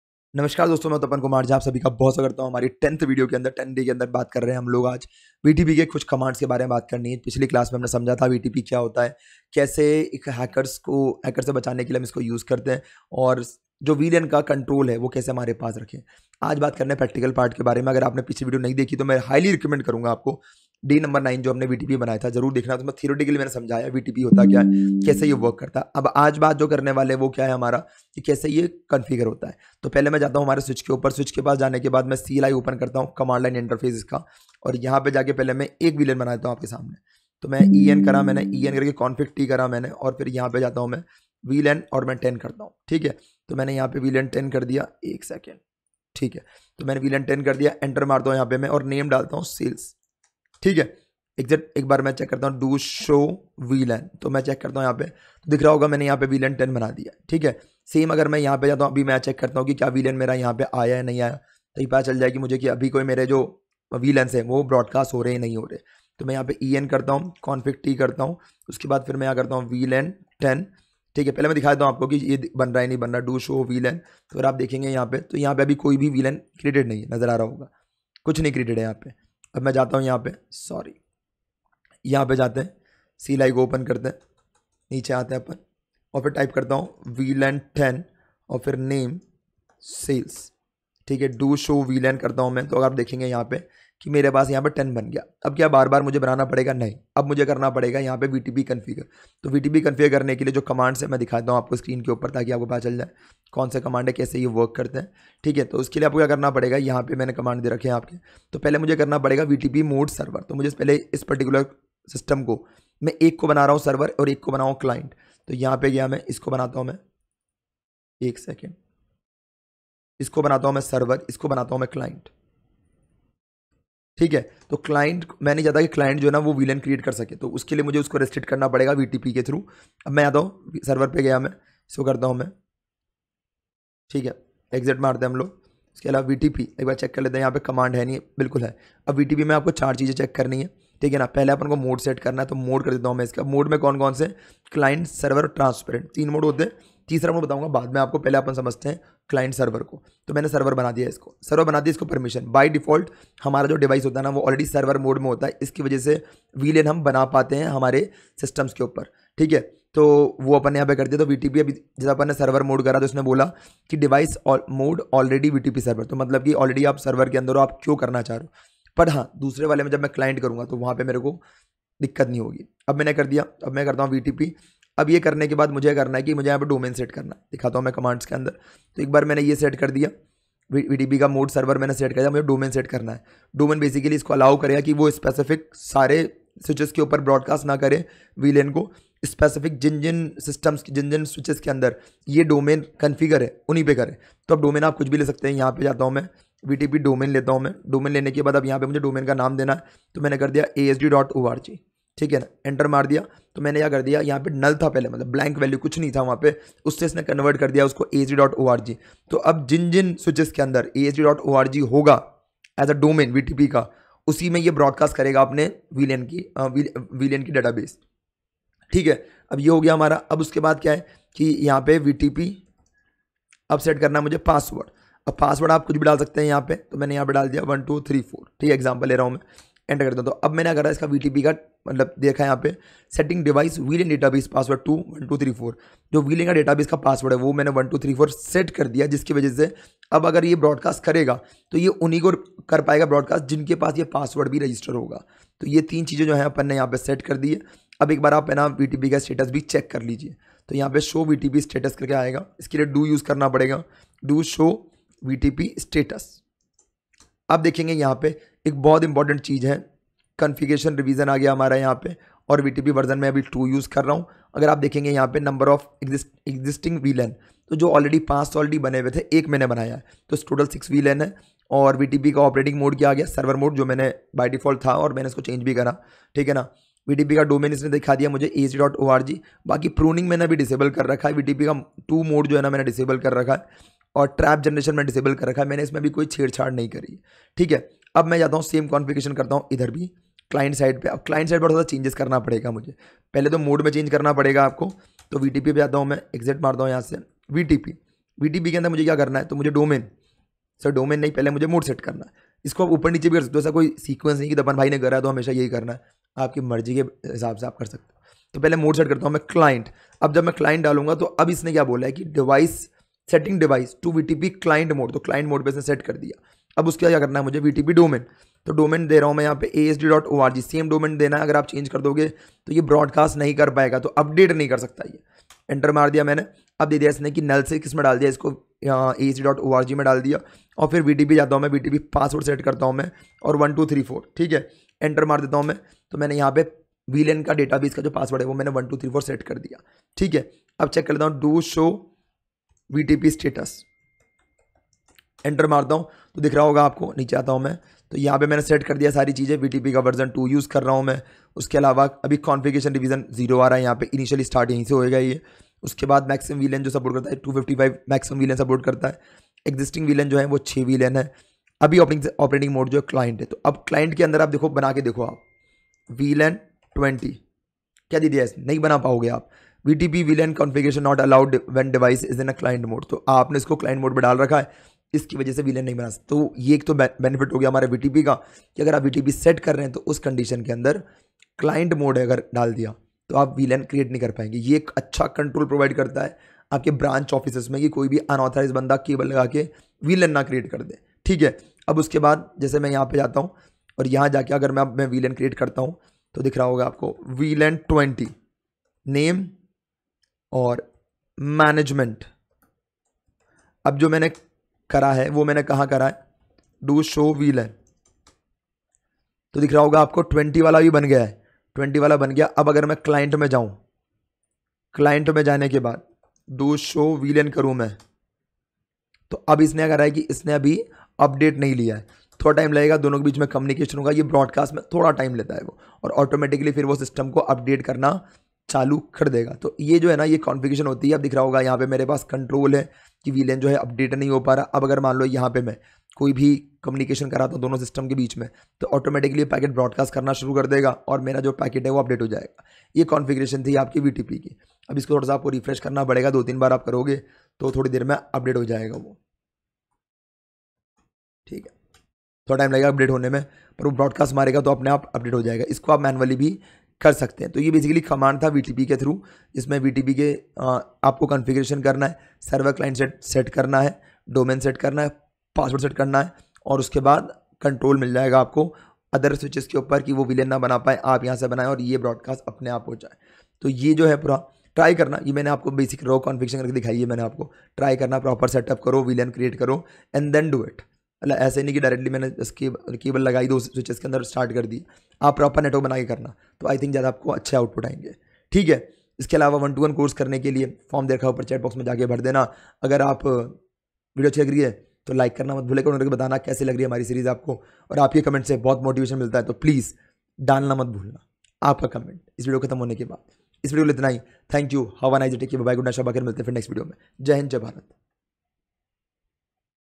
Day 10: [Practical] How to Configure VTP Step-by-Step | ccna full course cyber kaksha | cisco
In this video we will discuss about how to configure vtp and it's importance in the industry. If you are looking for ccna full course in hindi then this will be the best series for you where i will be covering full ccna courses routing and switching.